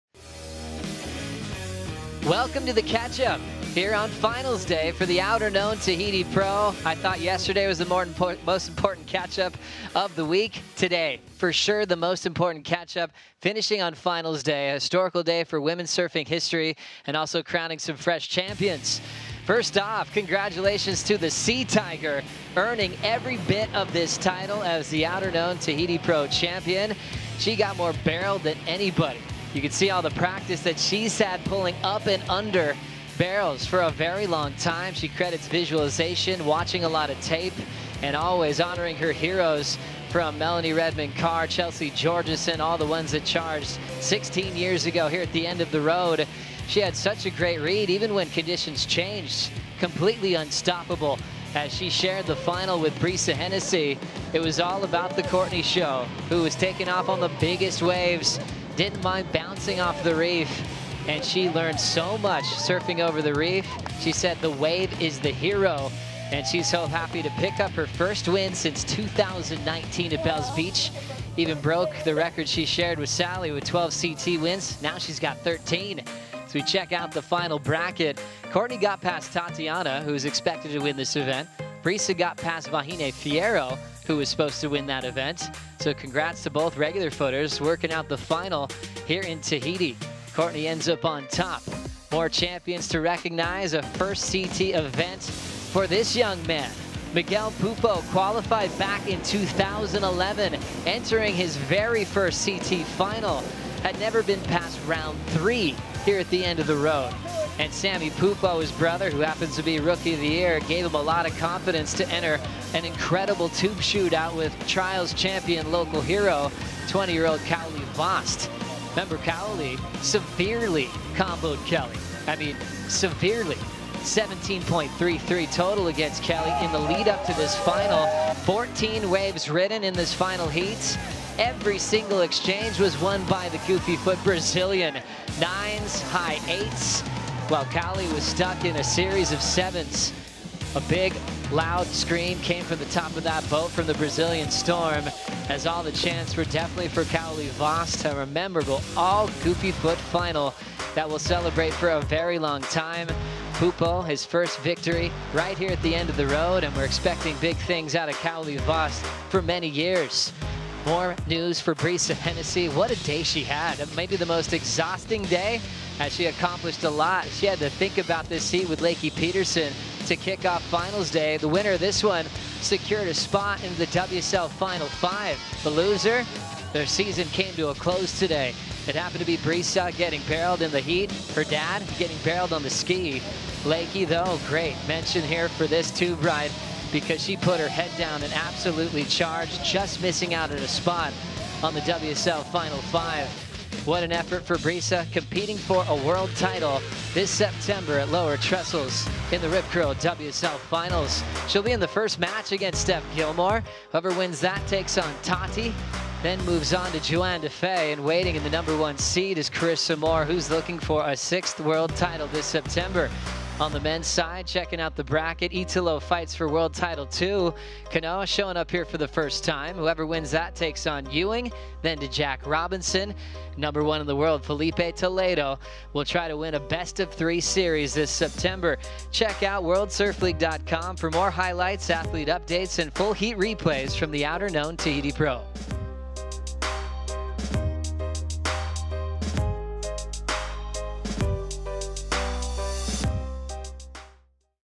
The Catch Up FINALS DAY: Women Throw Down The Gauntlet For Next Generation, Final 5 Solidified
Pupo claims breathtaking victory, Conlogue takes the day in generational inspiring moment for women's surfing, plus Hennessy, Igarashi prove themselves in the pressure cooker for Final 5 appearances.

The most critical wave in the world is now somehow more extreme than ever. Added to the mutant barrels, committed drops, and razor-sharp reef is the fact that Teahupo’o serves as the last fight for the WSL Final 5. The women get their first shot at the famed barrel in 16 years and the world’s best surfers get one last chance to lay it all on the line for a spot in the Rip Curl WSL Finals.